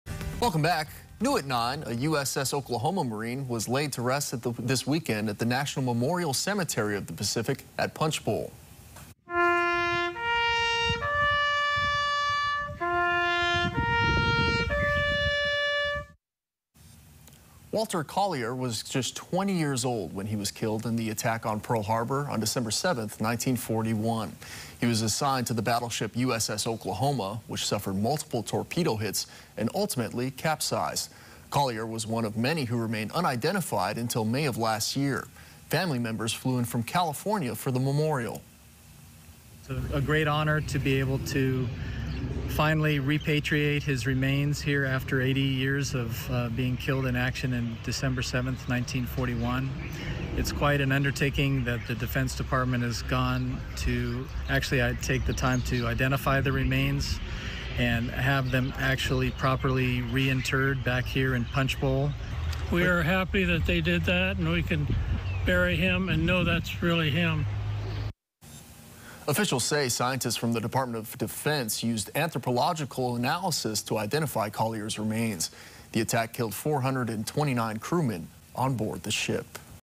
USS Oklahoma marine buried at National Memorial Cemetery of the Pacific
Marine Pfc. Walter Leon Collier, 20, of Burbank, California, from WWII was identified in May 2021 by the Defense POW/MIA Accounting Agency.